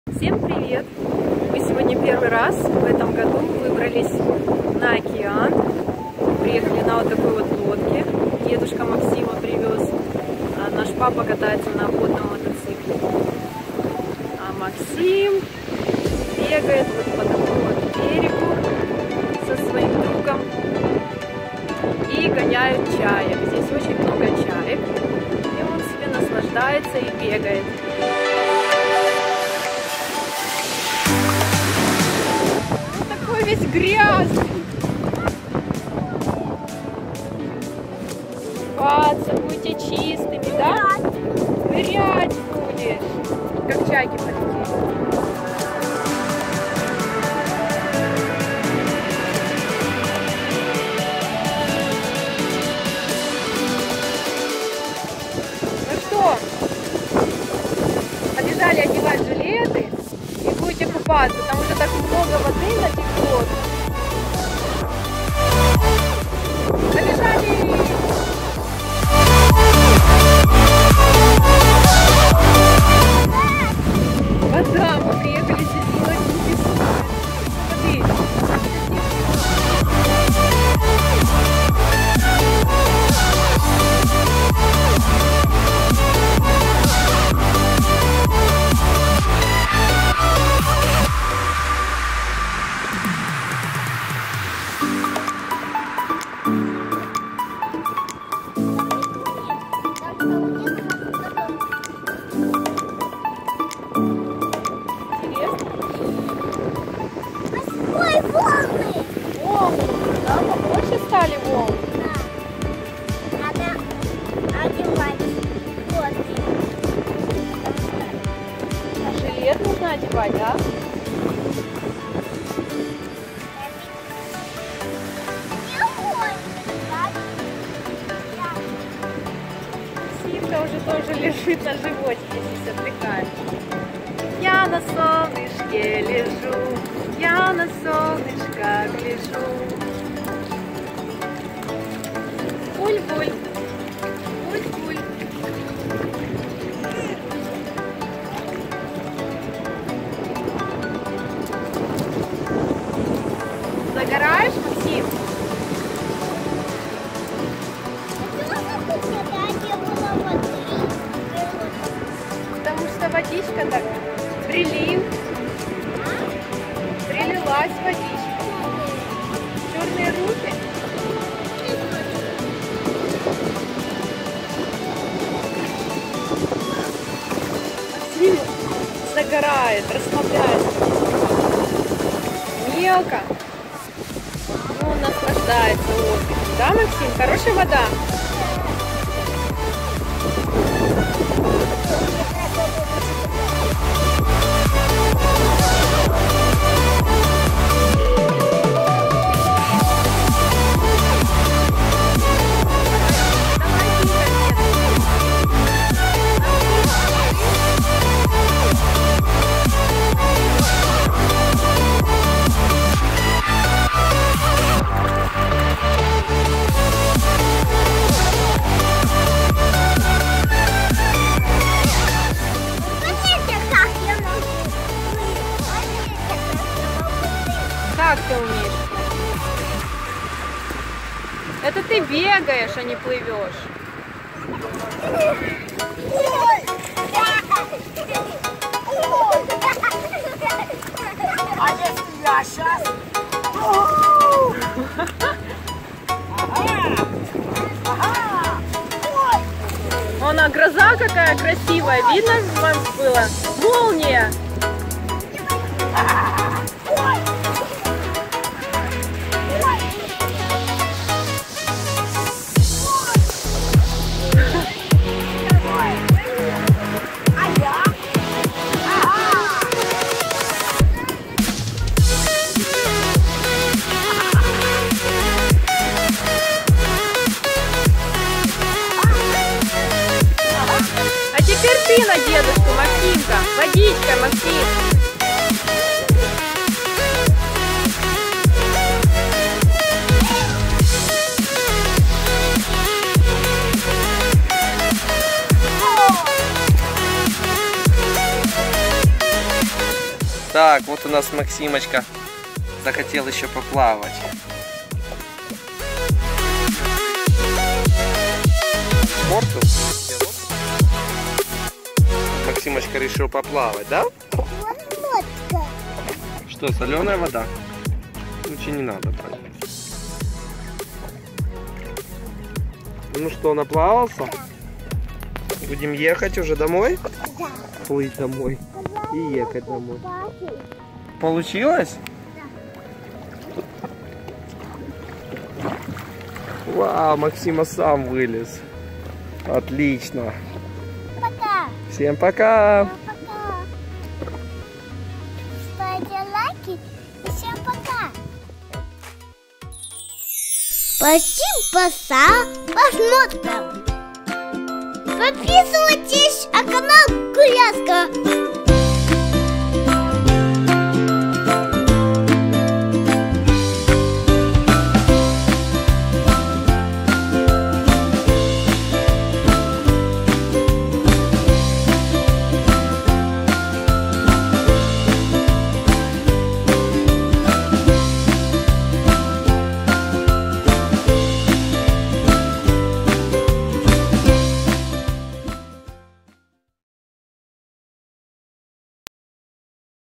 Максим Отдыхает на Океане Купается Играет с Другом и Прикалывается с Дедушкой Америка fun weekend
Максим первый раз за лето катается на лодке, водном мотоцикле (скутере) и купается в океане, загорает, бегает за чайками с другом, играется, плавает, прыскается водой с дедушкой, веселится и замечательно проводит выходной.

🌟 Предыдущее Видео Максим Помогает Родителям Развозить и Разгребать Землю для Сейки Газонной Травы Детский Влог